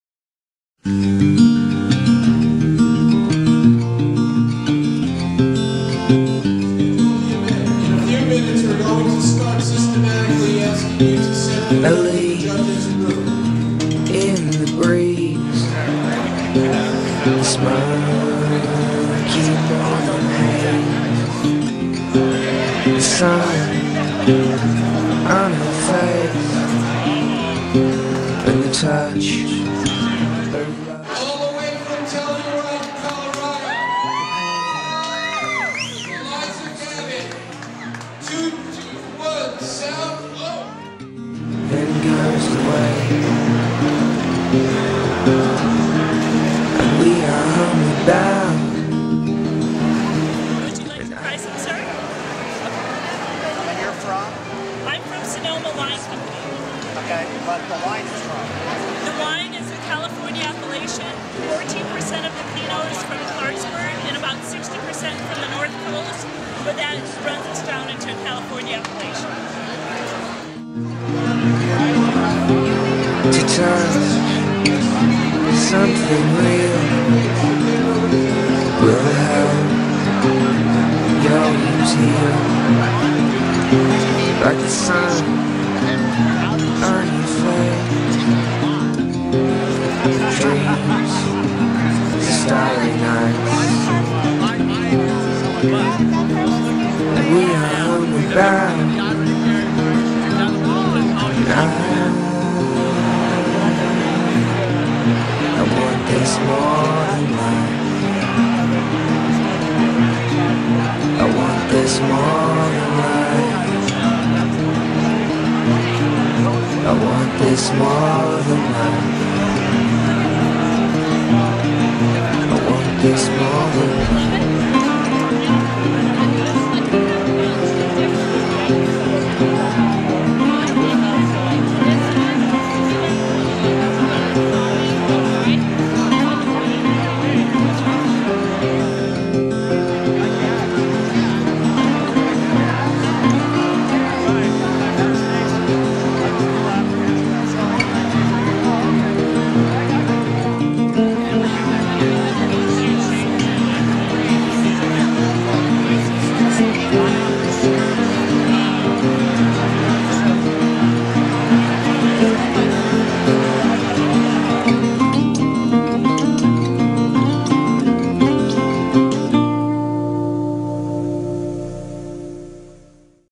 2010 Denver International Wine Festival
A short movie from The 2010 Denver International Wine Festival Taste Of Elegance Chefs Food and Wine Pairing Competition (11/04/10) and The Grand Tasting of International Wine & Food (11/05/10). Location: The Mile High Station, Denver, CO.  Denver's premier & first ever, International wine festival is produced and owned by Wine Country Network, Inc.